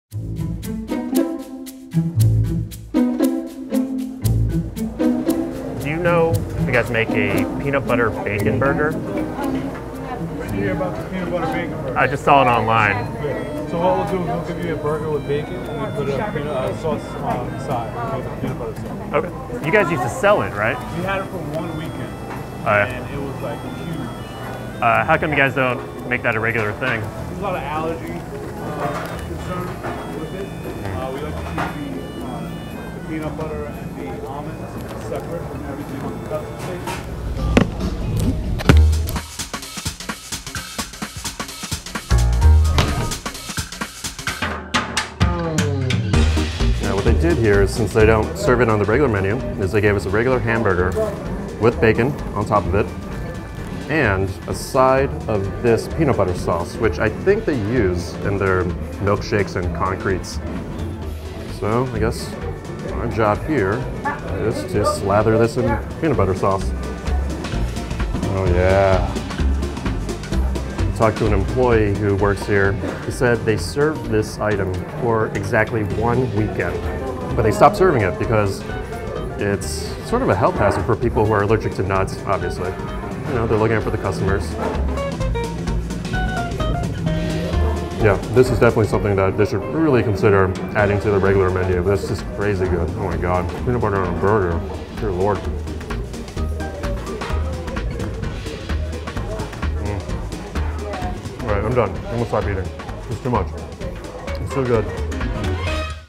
Shake Shack's secret menu burger
Despite its strong fanbase, not a lot of people know about a secret menu at Shake Shack. So we took a shot at ordering a secret menu item the burger chain once experimented with years ago. It's called the Peanut Butter Bacon Burger, and it needs to be brought back to life. Watch and find out why.